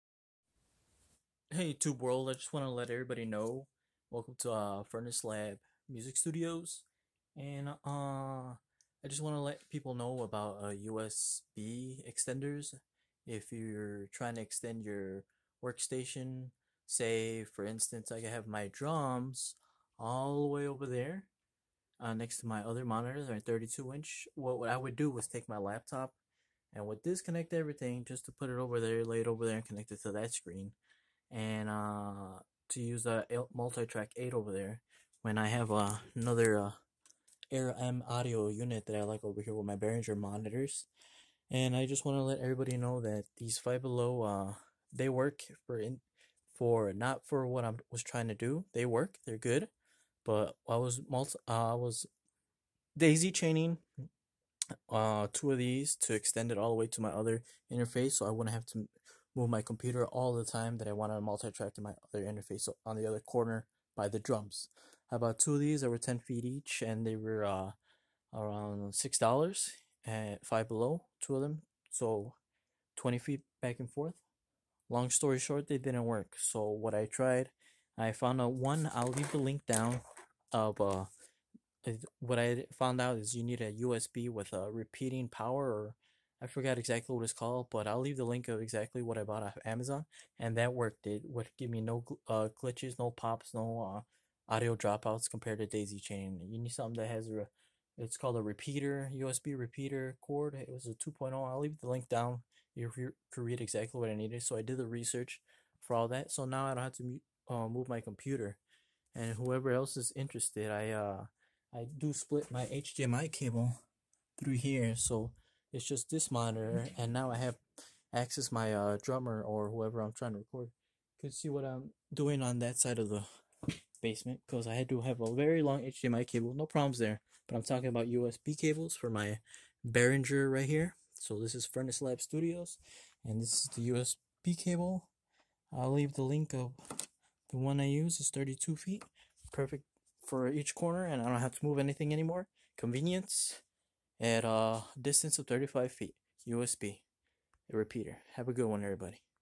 Audio interface USB Extending cable that works, don't daisy chain.
USB Extension Cable, TECHTOO® 32 Feet (10 Meter) USB 2.0 Active Cable Repeater Cable Type A Male to A Female with Built-in Signal Booster Chips